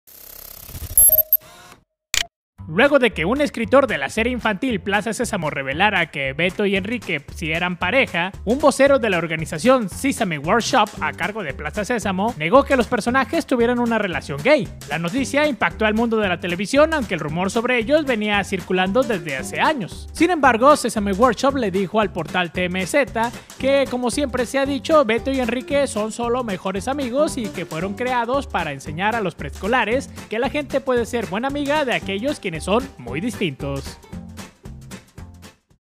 🌈Niegan que 'Beto' y 'Enrique' fueran pareja🌈
Un vocero de la organización Sesame Workshop, a cargo de Plaza Sésamo, negó que los personajes de "Beto" y "Enrique" sean una pareja gay.